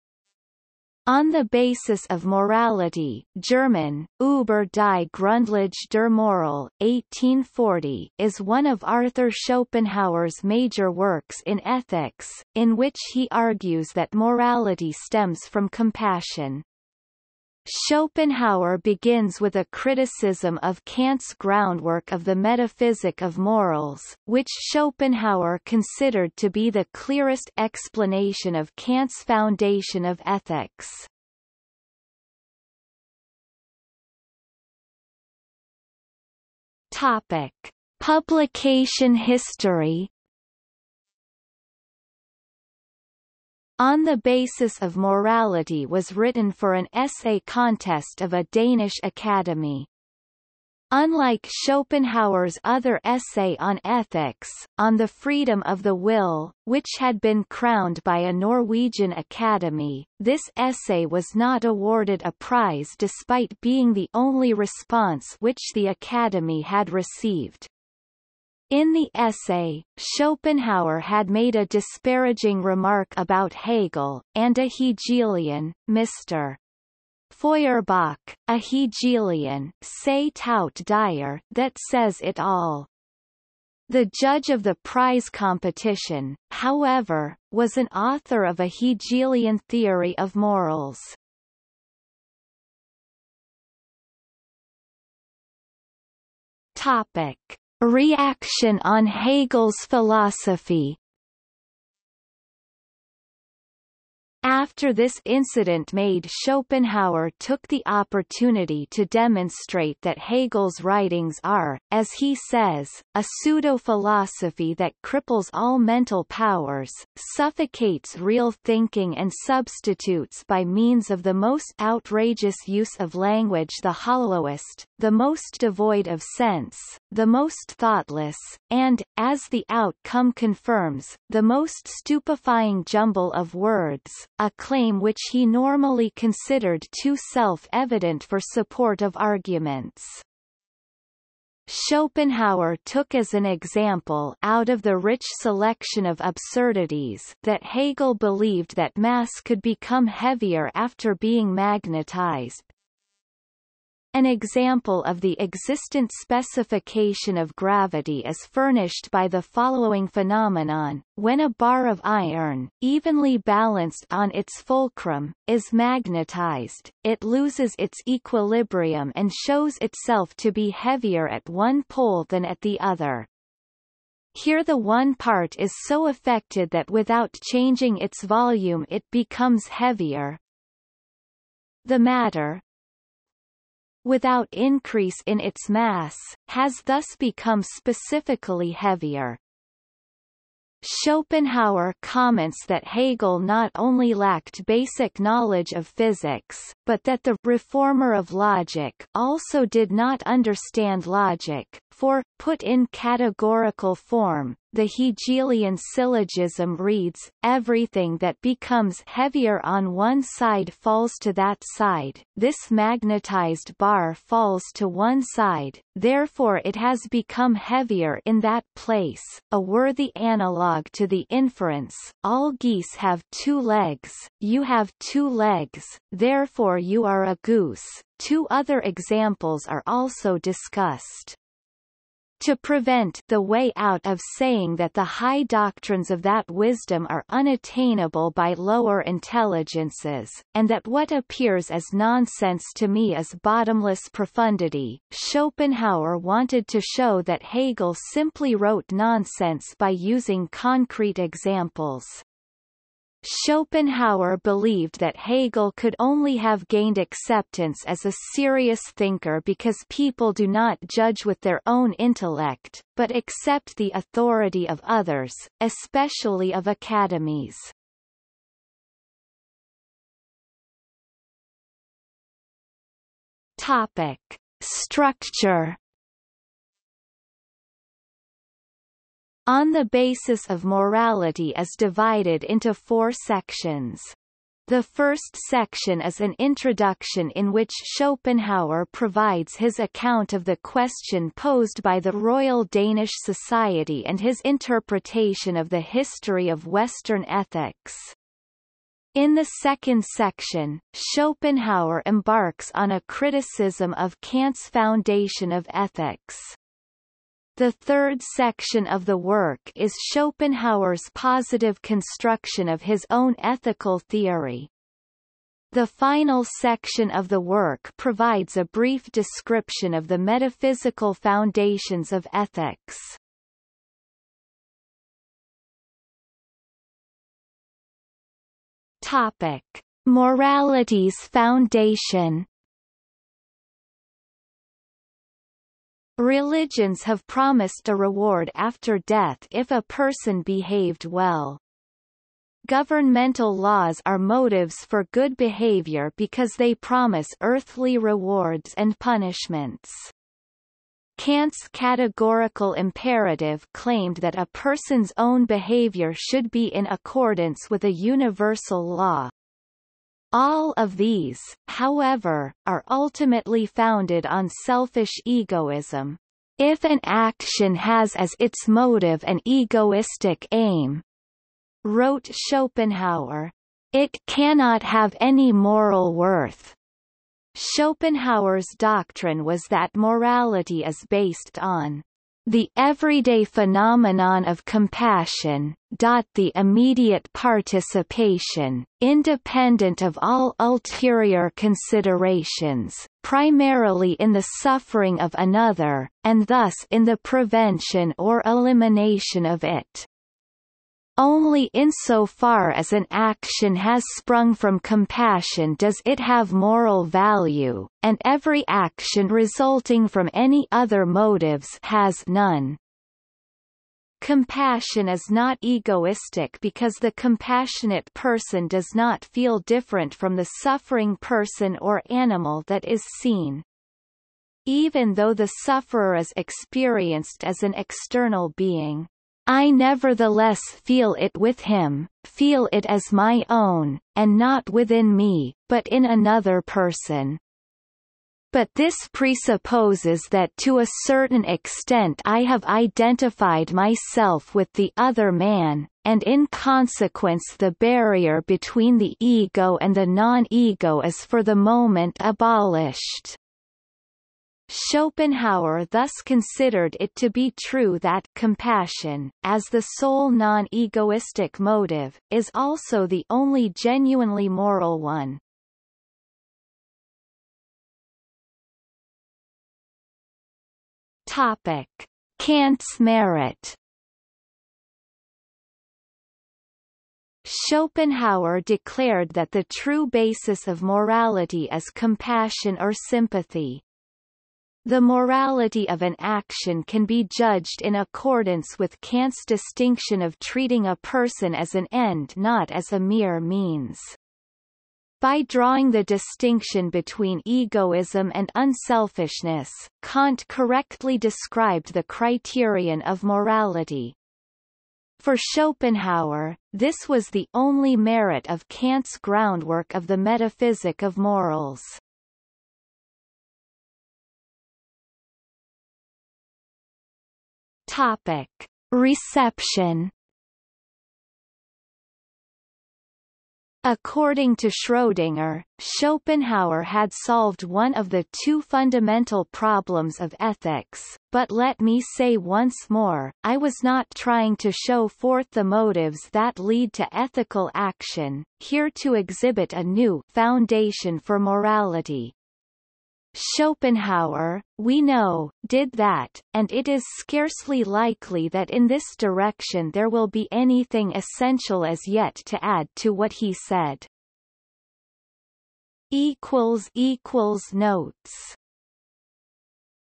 Schopenhauer's criticism of Kant's Groundwork of the Metaphysic of Morals | Wikipedia audio article
00:00:33 1 Publication history
00:01:33 1.1 Reaction on Hegel's philosophy
00:04:29 2 Structure
00:05:25 3 Morality's foundation
00:07:47 4 Kant's merit
00:08:34 5 Reception

- Socrates

SUMMARY
On the Basis of Morality (German: Ueber die Grundlage der Moral, 1840) is one of Arthur Schopenhauer's major works in ethics, in which he argues that morality stems from compassion. Schopenhauer begins with a criticism of Kant's Groundwork of the Metaphysic of Morals, which Schopenhauer considered to be the clearest explanation of Kant's foundation of ethics.